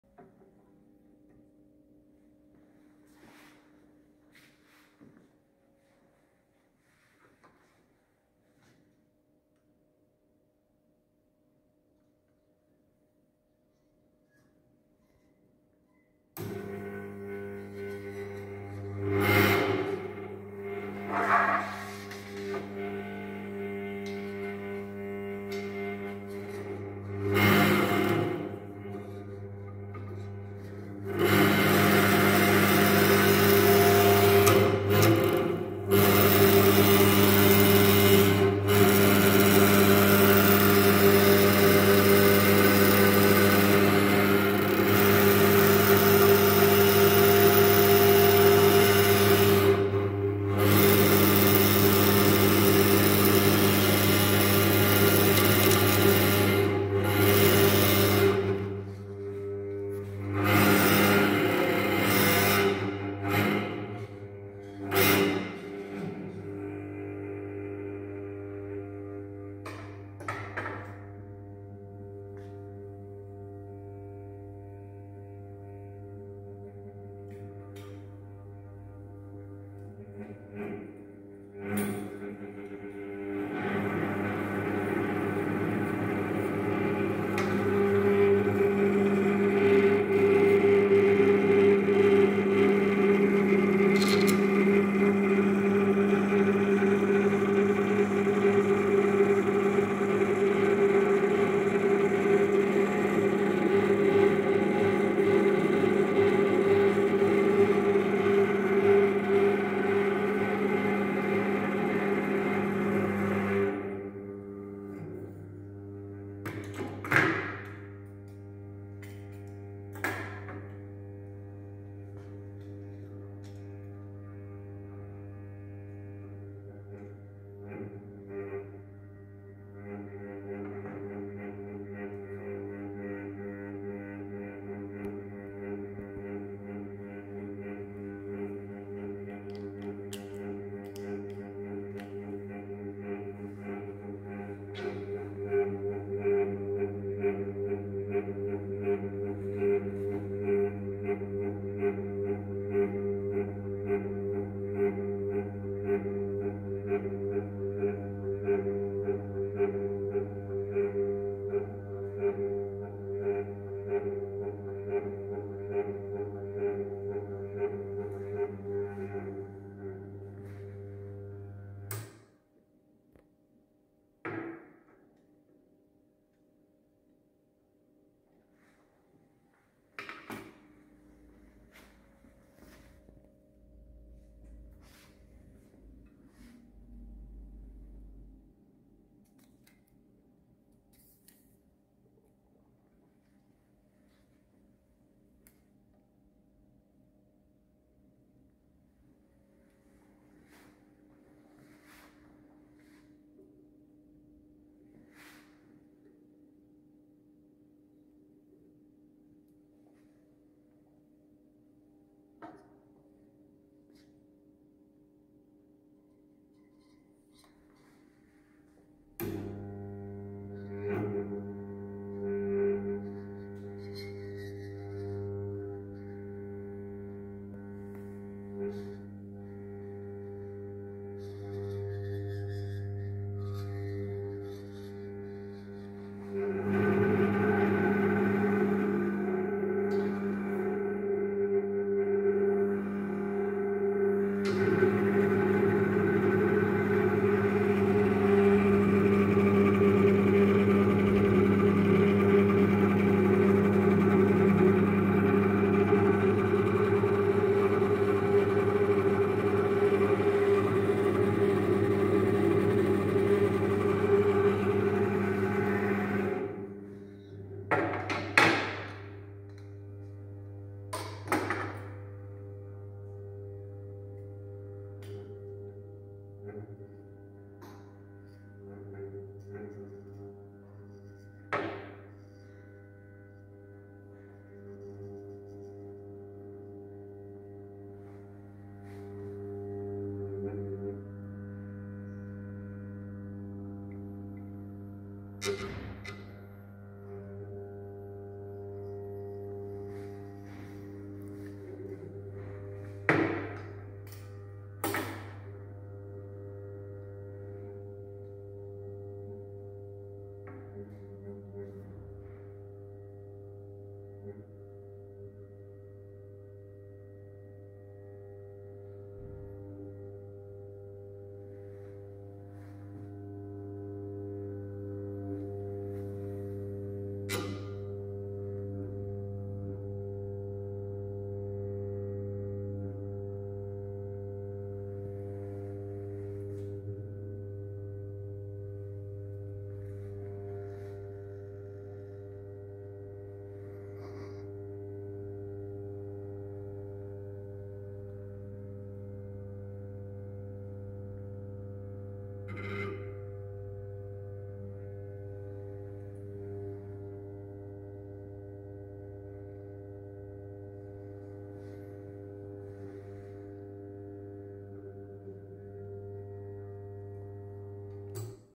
STOJECE VALOVANJE NA VRVI
Brnač ima konstantno frekvenco 100Hz.